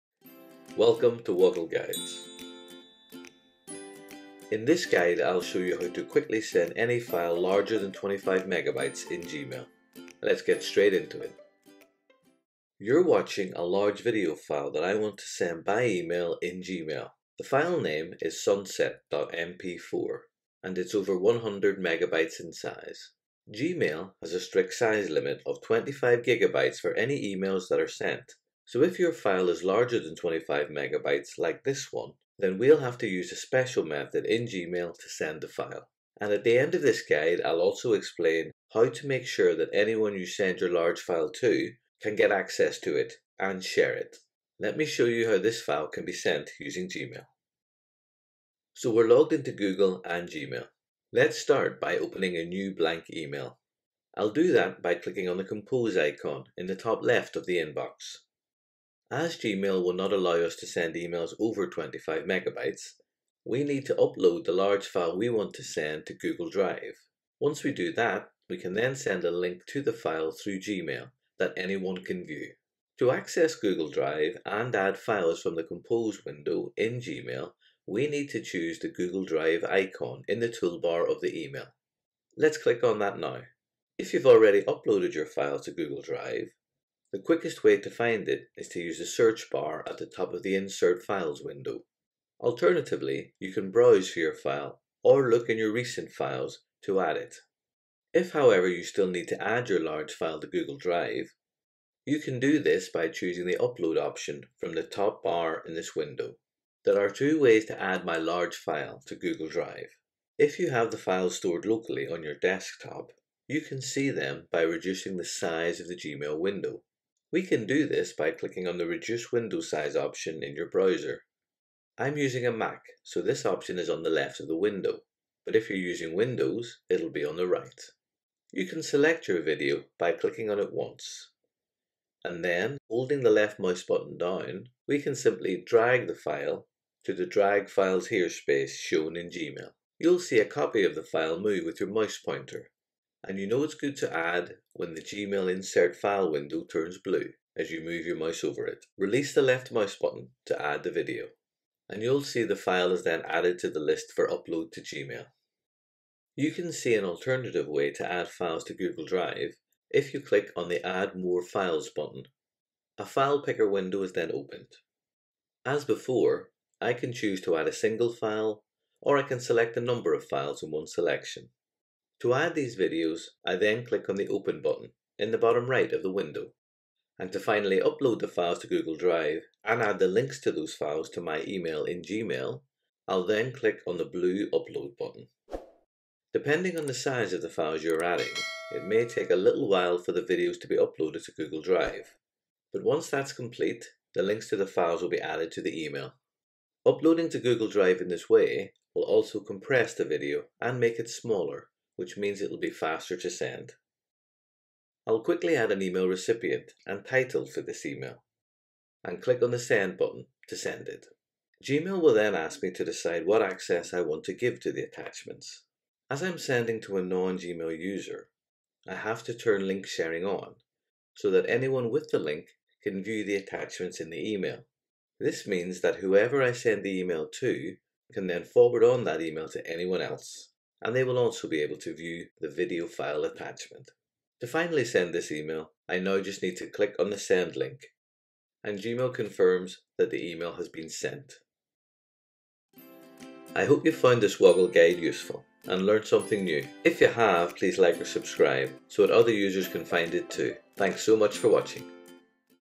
How to send a file bigger than 25mb on Gmail
Gmail has a maximum file size of 25MB that can be sent in an email.
In this guide, I'll show you how to quickly send any file larger than 25MB in Gmail

🕔  Key Moments
00:00 | Intro
00:47 | How to send files larger than 25MB in Gmail
04:03 | Wrap up

🔎  Useful links
See more info on how to send attachments of any sort in Gmail in this article from the Google Support Center ‘Send attachments with your Gmail message’